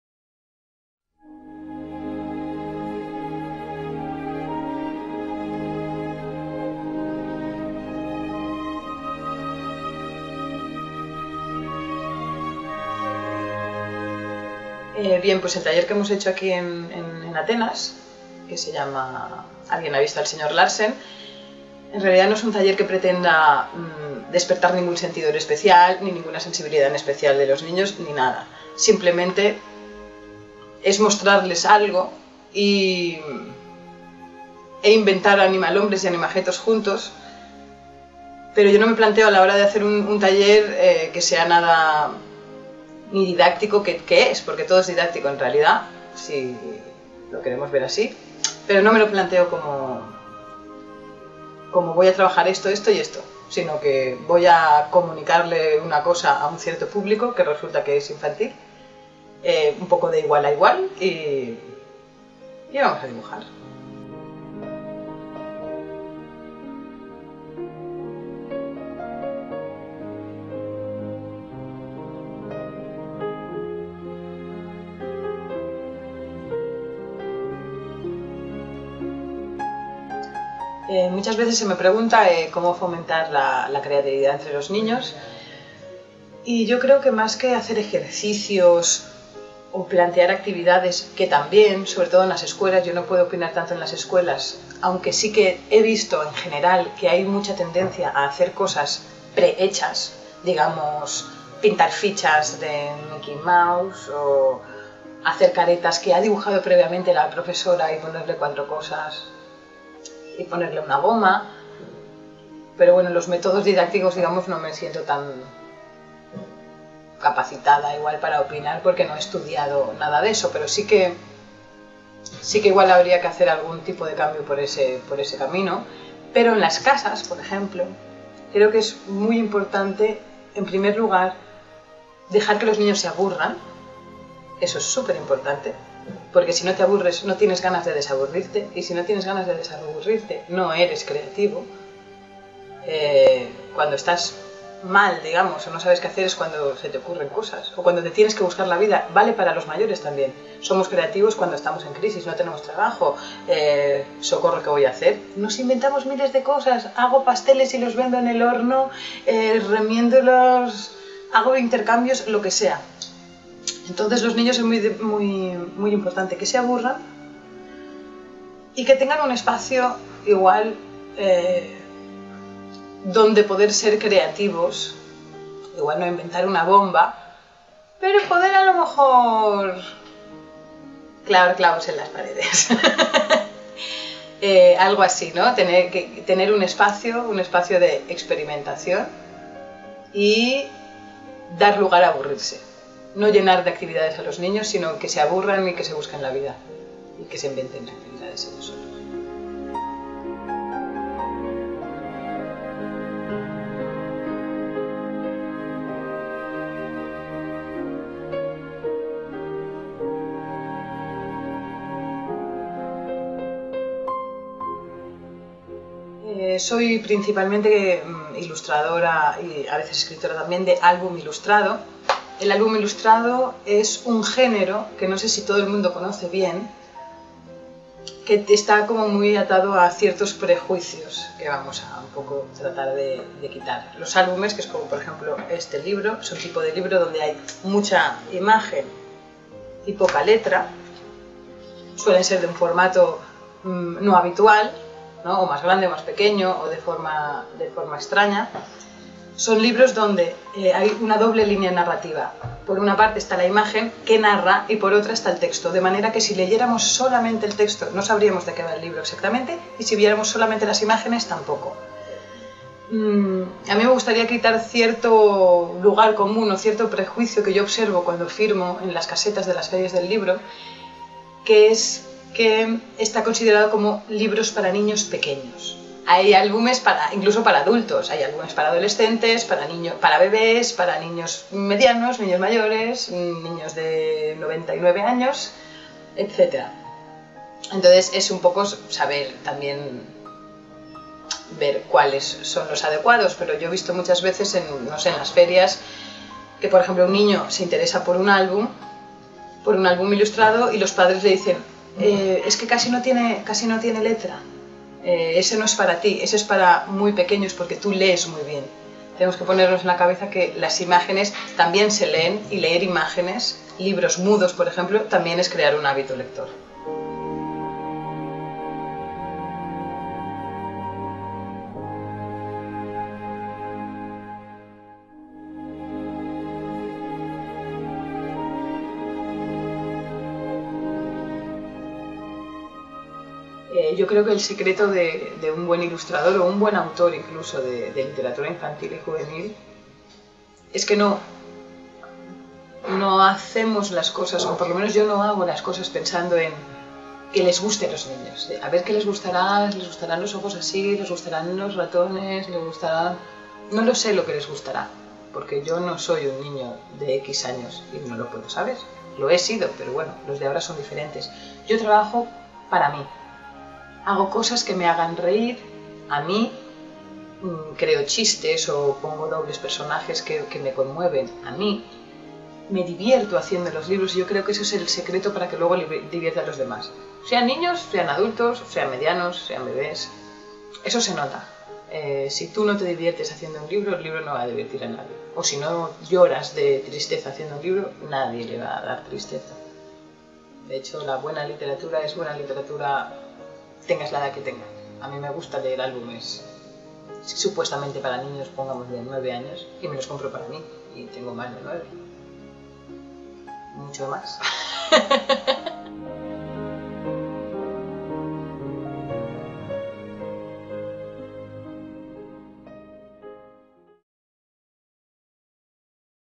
Aitana Carrasco en el Día de la Literatura Infantil en Atenas
Con motivo de la celebración del Día Cervantino de la Literatura Infantil y Juvenil en el Instituto Cervantes de Atenas, la ilustradora Aitana Carrasco nos habla en la biblioteca Juan Carlos Onetti de su trabajo, de los álbumes ilustrados y de cómo fomentar la creatividad en los niños.